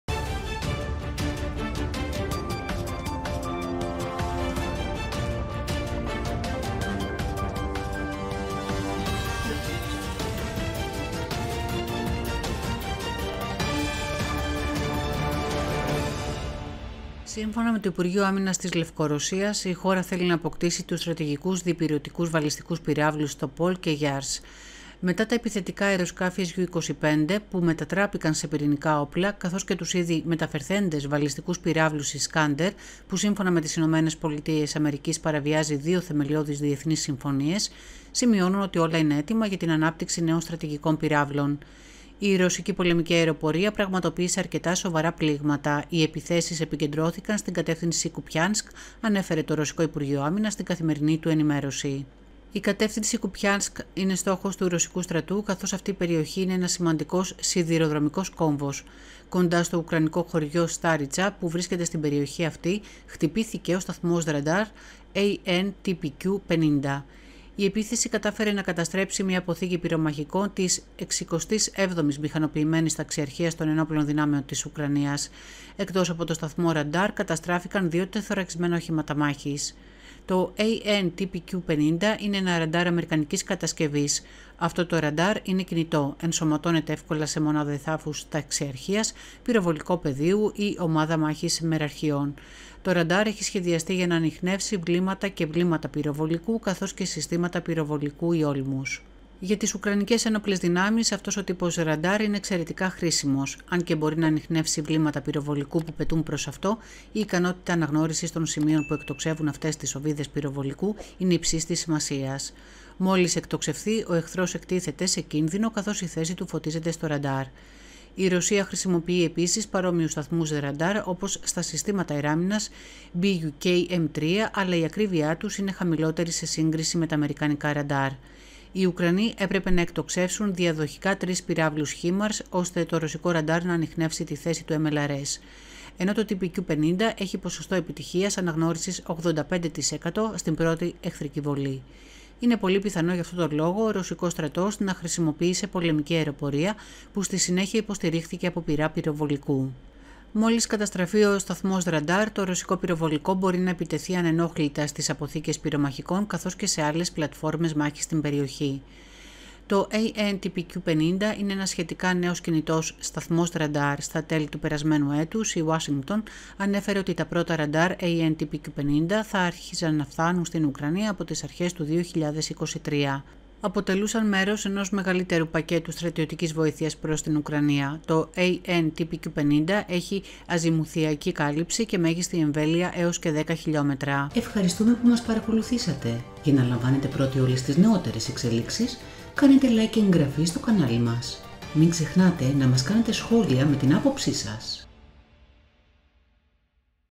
Οι Ρώσοι κατέστρεψαν αμερικάνικο ραντάρ AN/TPQ-50 ενώ στέλνουν στη Λευκορωσία πυραύλους Topol & Yars
Σύμφωνα με το Υπουργείο Άμυνας της Λευκορωσίας, η χώρα θέλει να αποκτήσει τους στρατηγικούς διηπειρωτικούς βαλλιστικούς πυραύλους Topol και Yars.

Η ρωσική πολεμική αεροπορία πραγματοποίησε αρκετά σοβαρά πλήγματα. «Οι επιθέσεις επικεντρώθηκαν στην κατεύθυνση Κουπιάνσκ», ανέφερε το ρωσικό υπουργείο Άμυνας στην καθημερινή του ενημέρωση.

Για τις ουκρανικές ένοπλες δυνάμεις, αυτός ο τύπος ραντάρ είναι εξαιρετικά χρήσιμος. Αν και μπορεί να ανιχνεύσει βλήματα πυροβολικού που πετούν προς αυτό, η ικανότητα αναγνώρισης των σημείων που εκτοξεύουν αυτές τις οβίδες πυροβολικού είναι υψίστης σημασίας. Μόλις εκτοξευθεί, ο εχθρός εκτίθεται σε κίνδυνο καθώς η θέση του «φωτίζεται» στο ραντάρ.

Κεφάλαια:
0:00 - Εισαγωγή
0:18 - Είδηση
0:42 - Αμερικανικός σταθμός ραντάρ AN/TPQ-50
1:39 - Σημαντικό για την άμυνα
2:39 - Παραδόσεις από το 2023
3:04 - Αποφώνηση

Μετά τα επιθετικά αεροσκάφη Su-25, που μετατράπηκαν σε πυρηνικά όπλα , καθώς και τους ήδη μεταφερθέντες βαλλιστικούς πυραύλους Iskanedr, που σύμφωνα με τις ΗΠΑ παραβιάζει δύο θεμελιώδεις διεθνείς συμφωνίες, σημειώνουν ότι όλα είναι έτοιμα για την ανάπτυξη νέων στρατηγικών πυραύλων.

Και η ουσία είναι ότι, η Λευκορωσία φέρεται να διατήρησε τις θέσεις τουλάχιστον του 56ου Συντάγματος πυραύλων, το οποίο είχε σταθμεύσει στην περιοχή του Σερέσεβο από τη δεκαετία του 1960. Αρχικά, ήταν οπλισμένο με πυραύλους R-12, στη δεκαετία του '80 έλαβε συστήματα μεσαίου βεληνεκούς RSD-10 Pioner-K και μετά την υπογραφή της Συνθήκης INF, μεταπήδησε στο RT-2PM Topol, που ήταν στη χώρα μέχρι το 1996. Η τοποθεσία αυτού του συντάγματος απέχει 27 χλμ. από τα σύνορα με την Πολωνία.

Αμερικανικός σταθμός ραντάρ όλμων AN/TPQ-50

Η κατεύθυνση Κουπιάνσκ είναι στόχος του ρωσικού στρατού, καθώς αυτή η περιοχή είναι ένας σημαντικός σιδηροδρομικός κόμβος. Κοντά στο ουκρανικό χωριό Στάριτσα, που βρίσκεται στην περιοχή αυτή, χτυπήθηκε ο σταθμός ραντάρ AN/TPQ-50. Η επίθεση κατάφερε να καταστρέψει μια αποθήκη πυρομαχικών της 67ης μηχανοποιημένης ταξιαρχίας των Ενόπλων Δυνάμεων της Ουκρανίας. Εκτός από τον σταθμό ραντάρ, καταστράφηκαν δύο τεθωρακισμένα οχήματα μάχης.

Το AN/TPQ-50 είναι ένα ραντάρ αμερικανικής κατασκευής. Αυτό το ραντάρ είναι κινητό. Ενσωματώνεται εύκολα σε μονάδα εδάφους ταξιαρχίας, πυροβολικό πεδίου ή ομάδα μάχης μεραρχιών. Το ραντάρ έχει σχεδιαστεί για να ανιχνεύει βλήματα και βλήματα πυροβολικού, καθώς και συστήματα πυροβολικού ή όλμους.

Σημαντικό για την άμυνα

Η Ρωσία χρησιμοποιεί επίσης παρόμοιους σταθμούς ραντάρ, όπως στα συστήματα αεράμυνας Buk-M3, αλλά η ακρίβειά τους είναι χαμηλότερη σε σύγκριση με τα αμερικανικά ραντάρ. Για παράδειγμα, ένας Ρώσος στρατιώτης επιβεβαίωσε στο ρωσικό μέσο Izvestnia ότι, οι Ουκρανοί έπρεπε να εκτοξεύσουν διαδοχικά τρεις πυραύλους HIMARS ώστε το ρωσικό ραντάρ να παρακολουθεί την πορεία των πυραύλων, ανιχνεύοντας έτσι τη θέση του MLRS. Ενώ το TPQ-50 έχει ποσοστό επιτυχίας αναγνώρισης 85% στην πρώτη εχθρική βολή.

Είναι πολύ πιθανό για αυτόν τον λόγο,  ο ρωσικός στρατός να χρησιμοποίησε πολεμική αεροπορία, που στη συνέχεια υποστηρίχθηκε από πυρά πυροβολικού. Μόλις καταστραφεί ο σταθμός ραντάρ, το ρωσικό πυροβολικό μπορεί να επιτεθεί ανενόχλητα στις αποθήκες πυρομαχικών καθώς και σε άλλες πλατφόρμες μάχης στην περιοχή.

Παραδόσεις από το 2023

Το AN/TPQ-50 είναι ένας σχετικά νέος κινητός σταθμός ραντάρ. Στα τέλη του περασμένου έτους, η Ουάσιγκτον ανέφερε ότι τα πρώτα ραντάρ AN/TPQ-50 θα άρχιζαν να φτάνουν στην Ουκρανία από τις αρχές του 2023. Αποτελούσαν μέρος ενός μεγαλύτερου πακέτου στρατιωτικής βοήθειας προς την Ουκρανία. Το AN/TPQ-50 έχει αζιμουθιακή κάλυψη και μέγιστη εμβέλεια έως και 10 km.